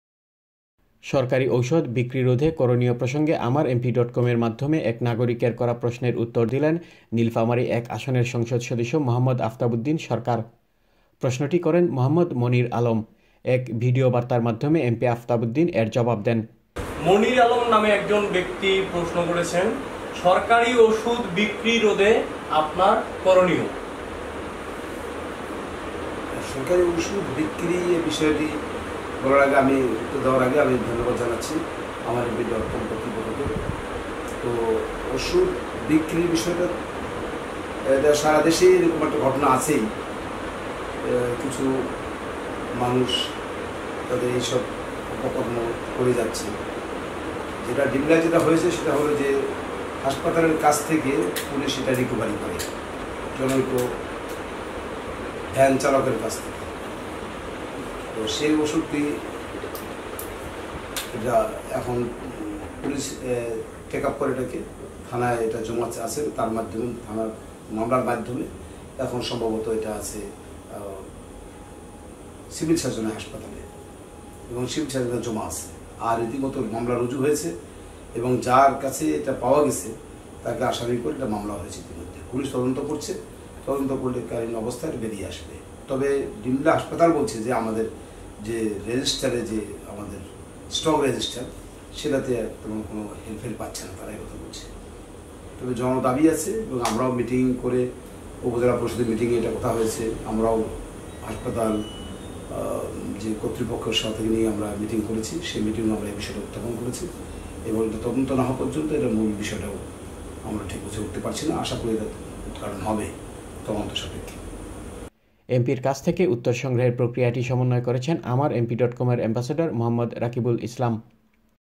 Md. Aftab Uddin Sarker replied to MP AmarMP.com regarding misuse of Govt. Medicine
সরকরী ঔষুধ বিক্রি রোধে করনীয় প্রসঙ্গে আমার এমপি ডট কমের মাধ্যমে এক নাগরিকের করা প্রশ্নের উত্তর দিলেন নীলফামারী ১ আসনের সংসদ সদস্য মোঃ আফতাব উদ্দিন সরকার। প্রশ্নটি করেন  মোঃ মনির আলম। এক ভিডিও বার্তার মাধ্যমে এমপি আফতাব উদ্দিন এর জবাব দেন।

এমপির কাছ থেকে উত্তর সংগ্রহের প্রক্রিয়াটি সমন্বয় করেছেন  আমার এমপি ডট কমের এম্বাসেডর মোঃ রাকিবুল ইসলাম।